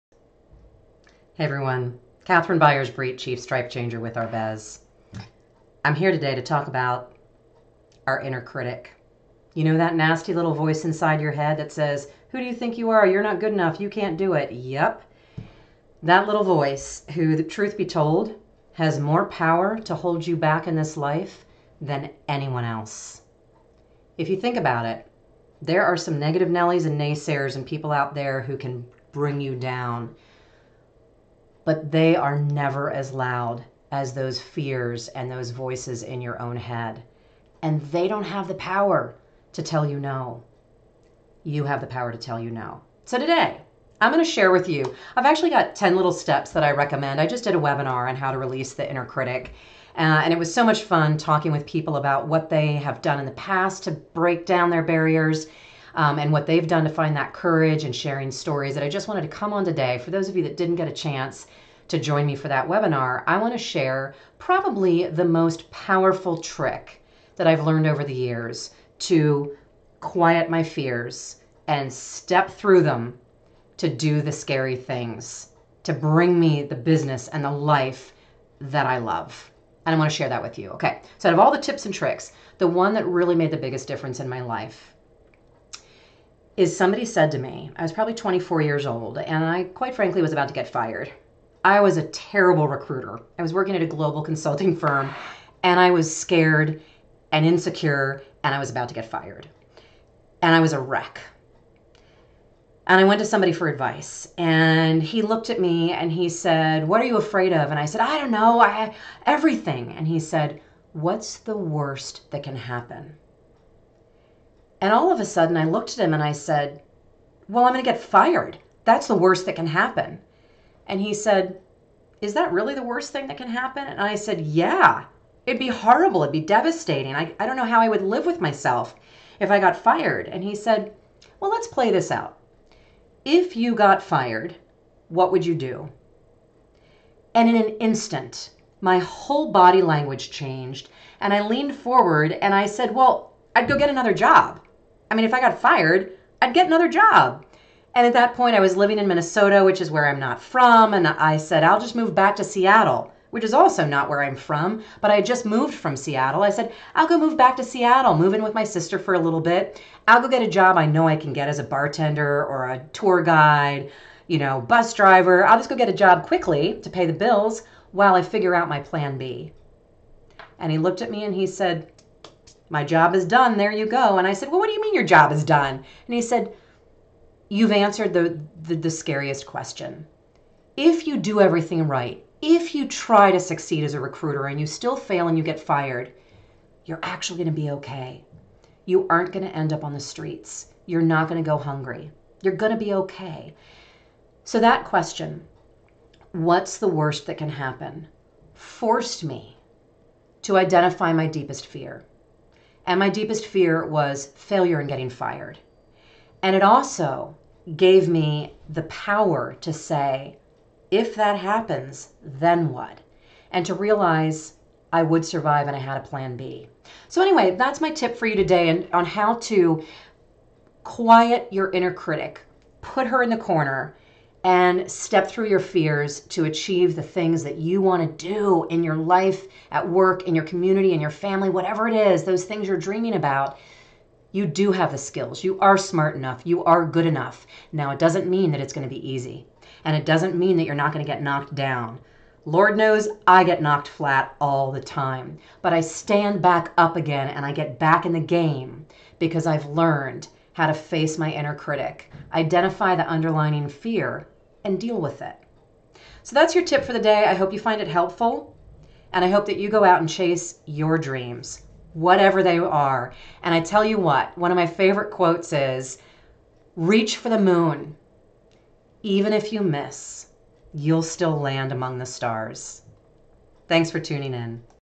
Release your Inner Critic! Here's how.
The voice in my head can be a real jerk! Honestly. She is nastier than anyone else in my life AND she knows all the right buttons to push. For years, I let her hold me back. And then one day, I was forced to make a decision: run away from the scary stuff ... yet again ... or face my fears, stand up and take a chance. The first time I chose door #2, my life changed dramatically. I not only kept my job, but I excelled. Two years later, I got promoted and doubled my income. Life on the other side of fear is amazing ... but first you've got to figure out how to face that inner critic. In this video, I'll give you my favorite strategy for winning the conversation with her.